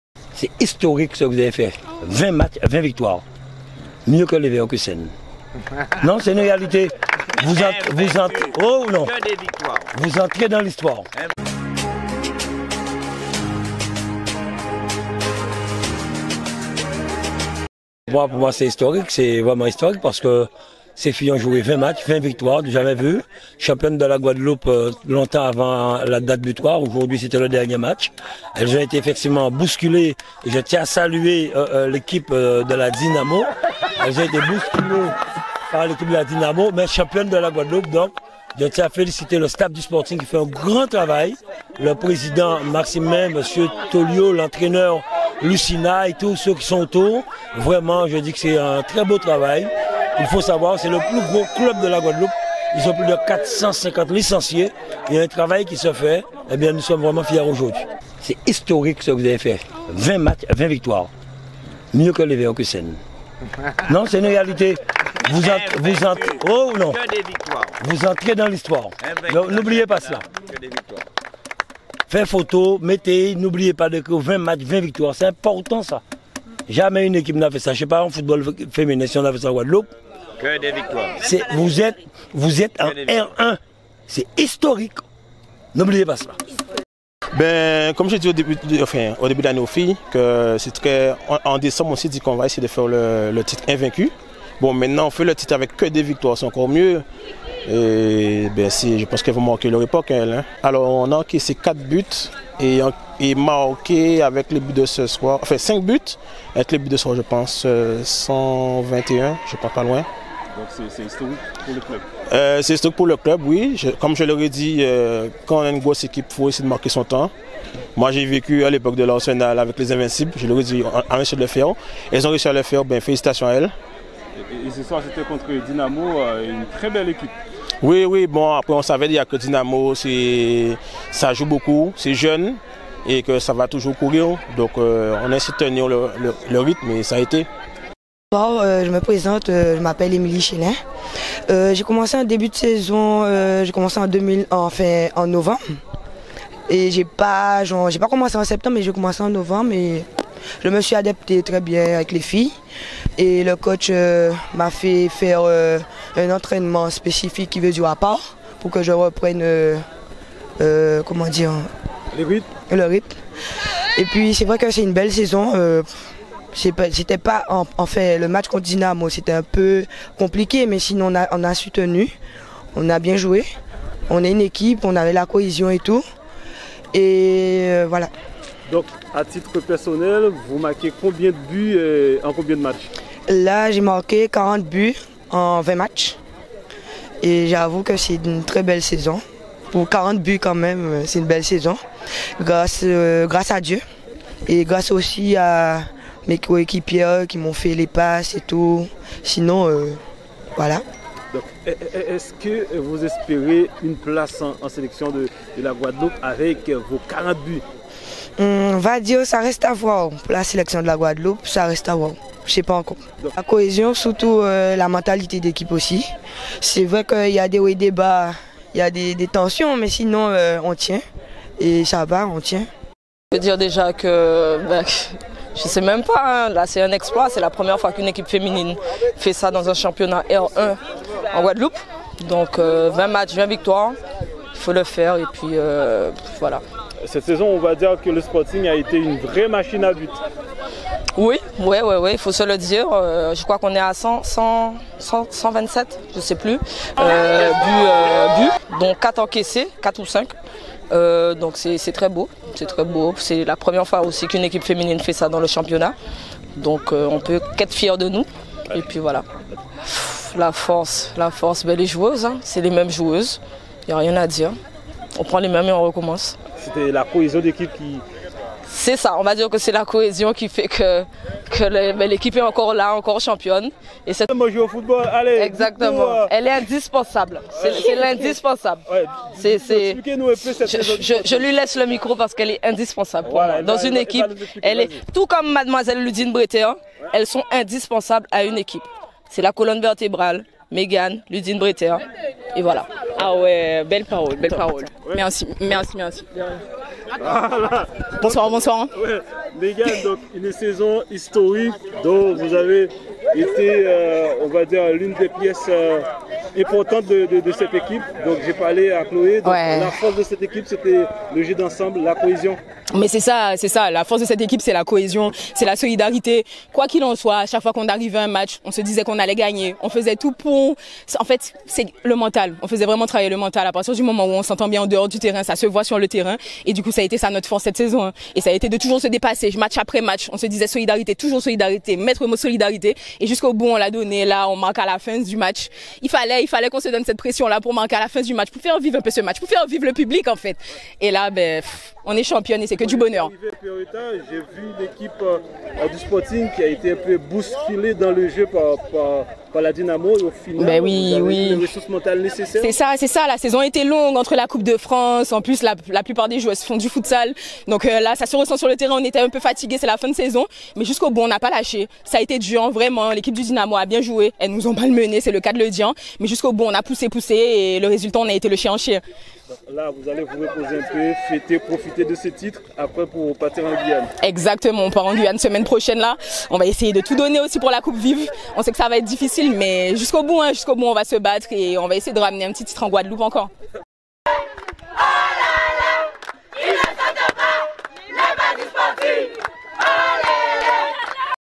SPORTING CHAMPION FEMININE
L'équipe Féminine du SPORTING remporte la R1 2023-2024 INVAINCUE